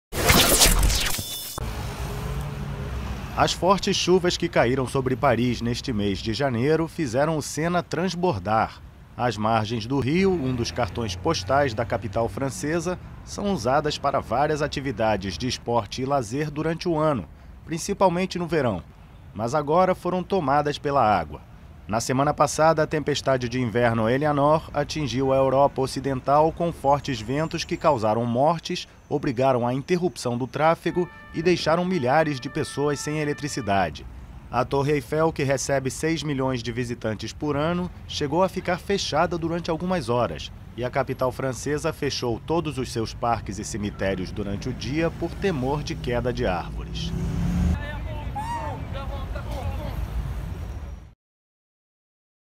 Chuvas fazem rio Sena transbordar
O Rio Sena, em Paris, transbordou devido às fortes chuvas que caem sobre a capital francesa em janeiro. As margens do rio, muito usadas para o lazer e a prática de esportes, foram tomadas pela água.